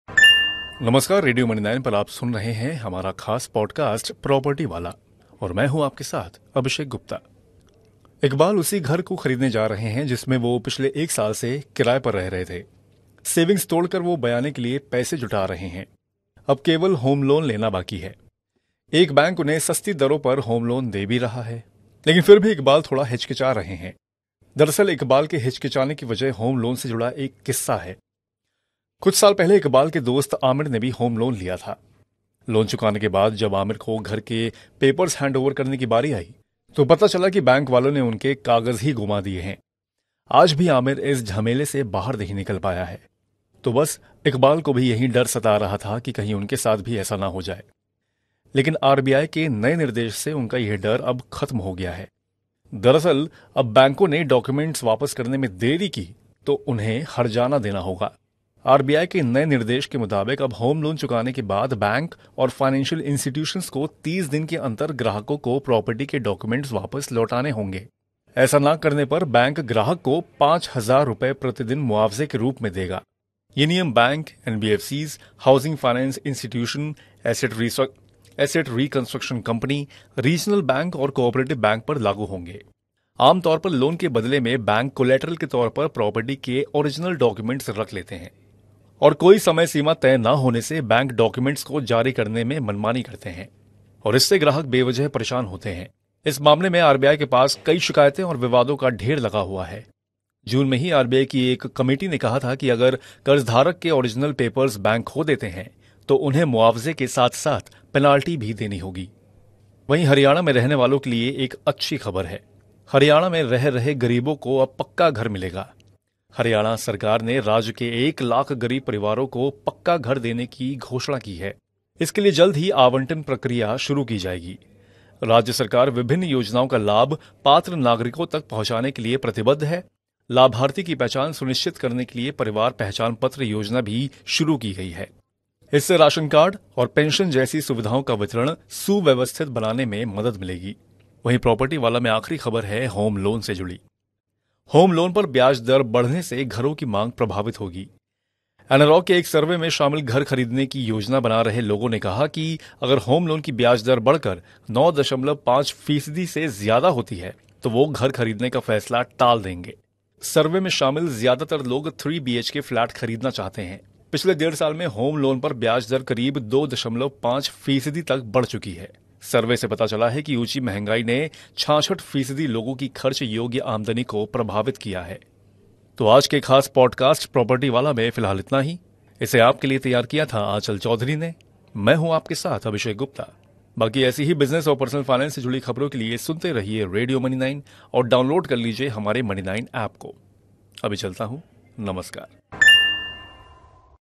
बैंकों को किस लिए देना पड़ेगा हर्जाना? | Podcast
होम लोन लेने से डर रहे इकबाल को RBI के किस निर्देश से मिली राहत? रिजर्व बैंक ने ऐसा क्या निर्देश दिया बैंकों को? सुनिए प्रॉपर्टीवाला अभिषेक गुप्ता के साथ.